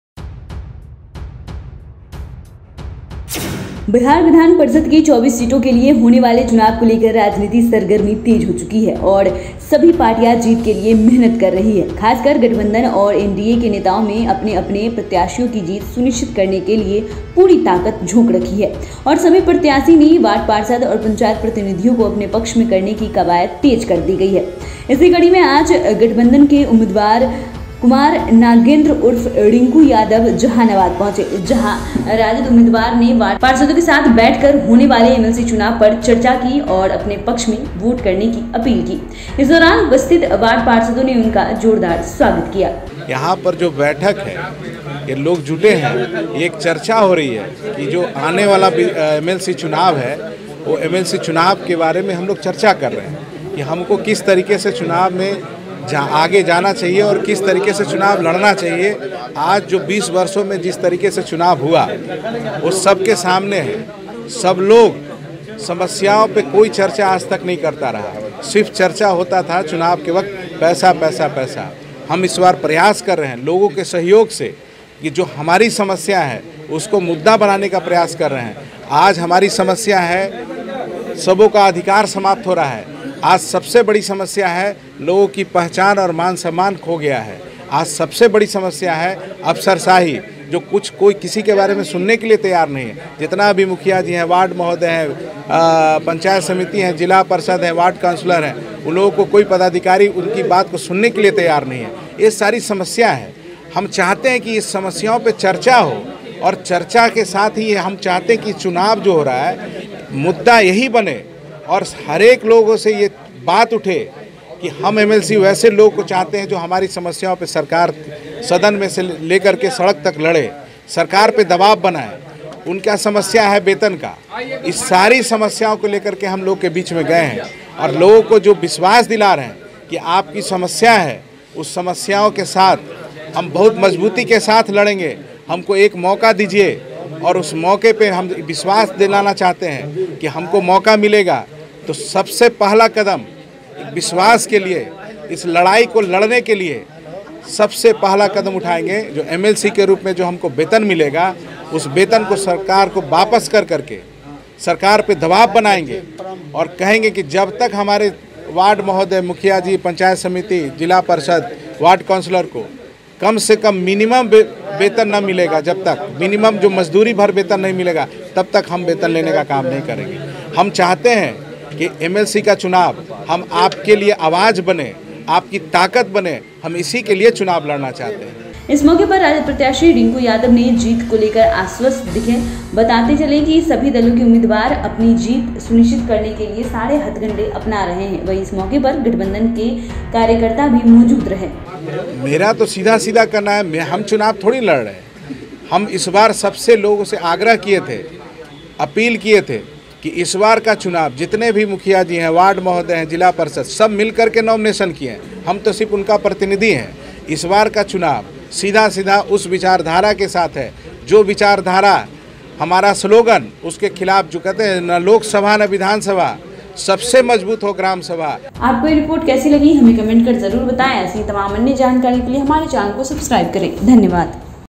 Jehanabad में MLC Chunav को लेकर सरगर्मी तेज, RJD उम्मीदवार ने कही ये बात, कार्यकर्ता भी रहे मौजूद..
Jehanabad में MLC Chunav को लेकर सरगर्मी तेज, RJD उम्मीदवार ने कही ये बात, कार्यकर्ता भी रहे मौजूद.. | Bihar News | Bolta Bihar